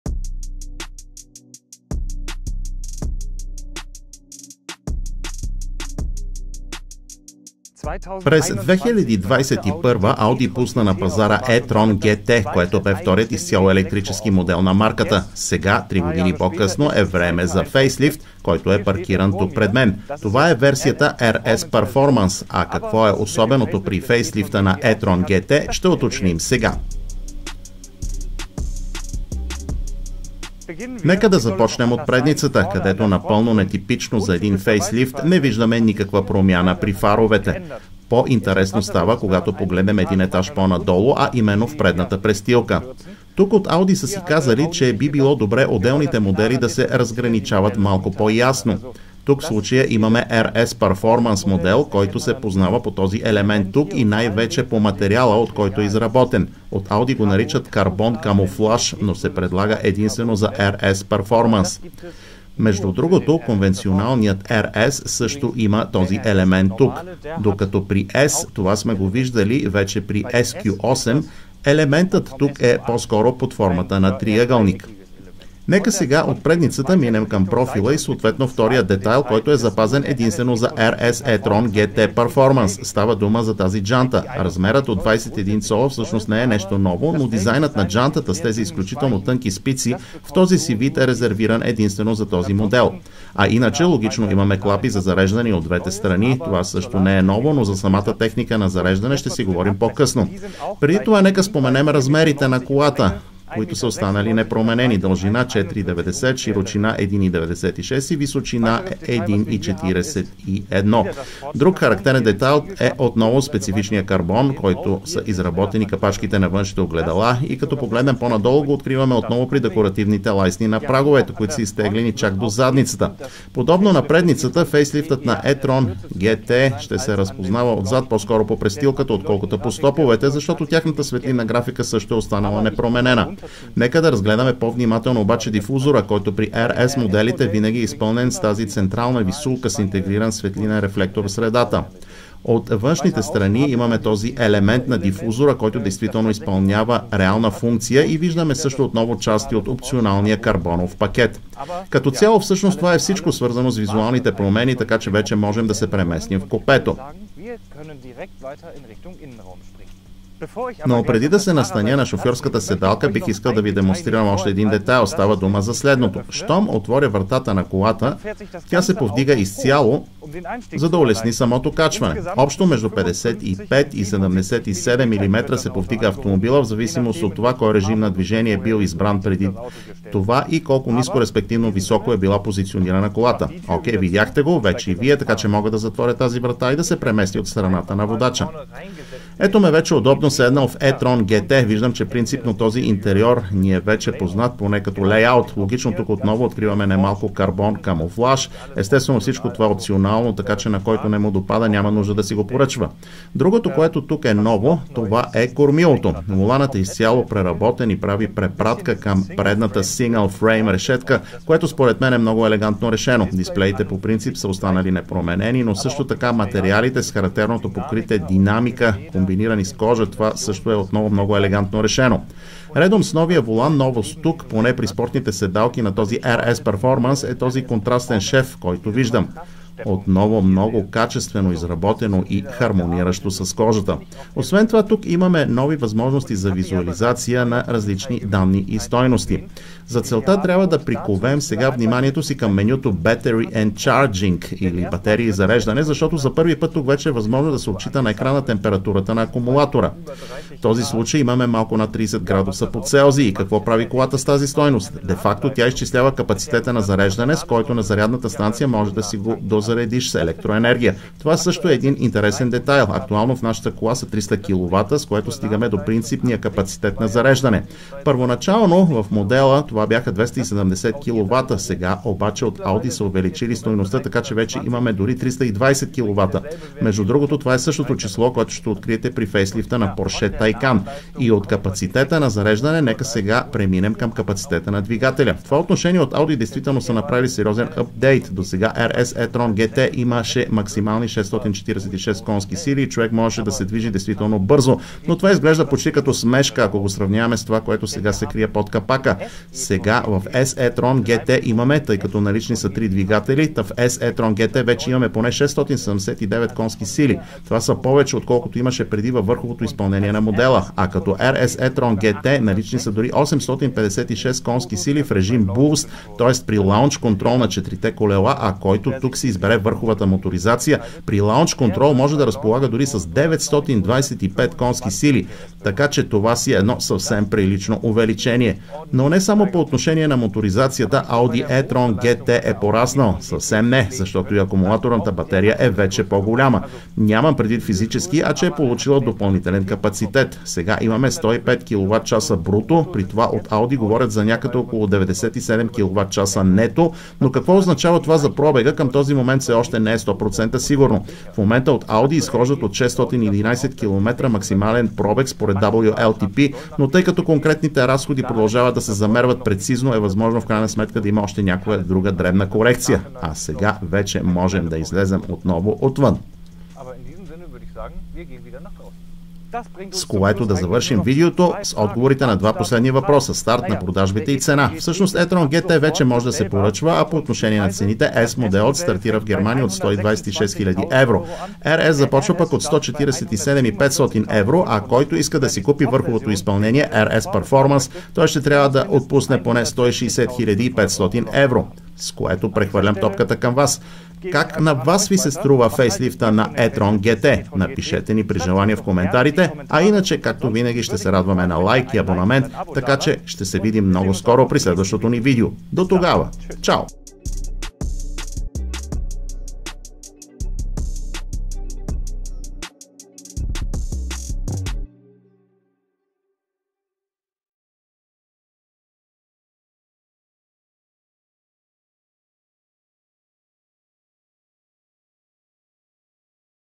RS Performance: най-потентата версия на ракетата Audi e-tron GT
През 2021 Аudi пусна на пазара e-tron GT, което бе вторият изцяло електрически модел на марката. Сега, три години по-късно е време да фейслифт, който е паркиран пред мен. Това е версията RS Performance. А какво е особеното при фейслифта на e-tron GT ще уточним сега.

@AUTO BILD Bulgaria